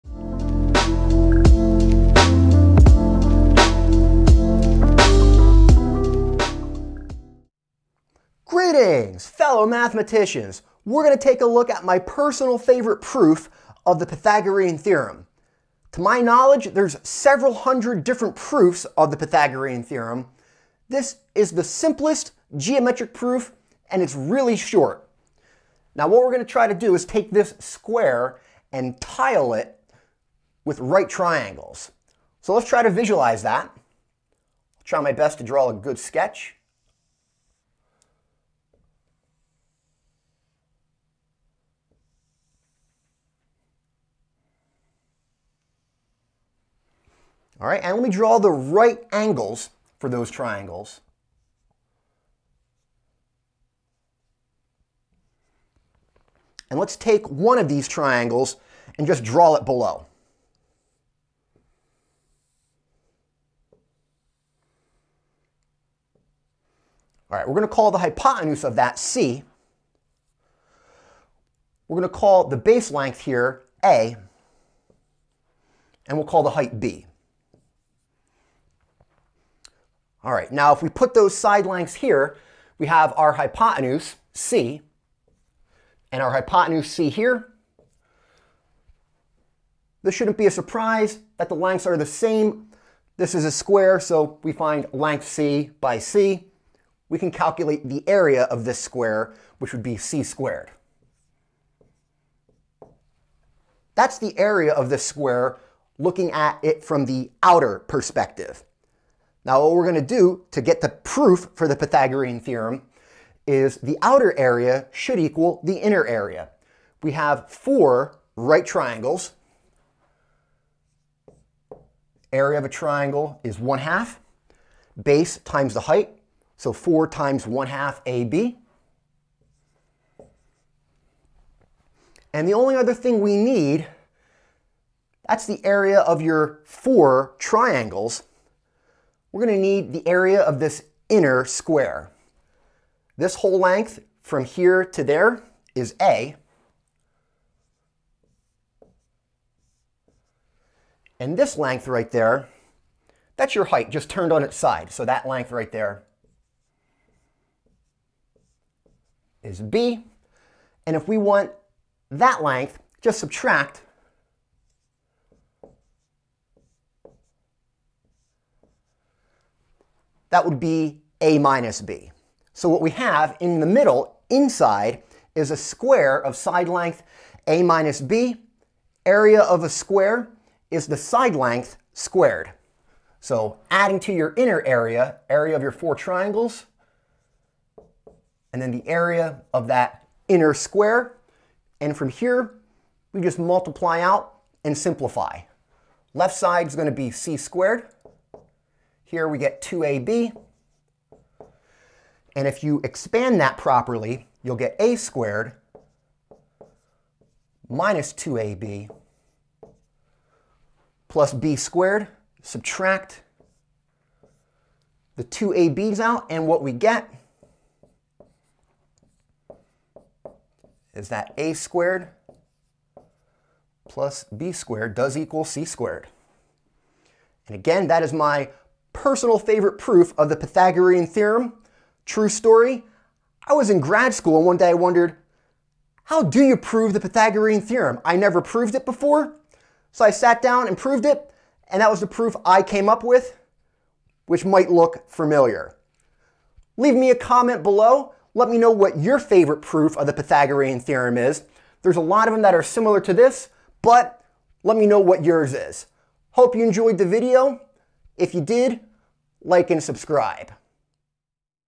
Proof of the Pythagorean Theorem
This is one of my favorite proofs in all of mathematics. We prove the Pythagorean Theorem using a simple, geometric construction.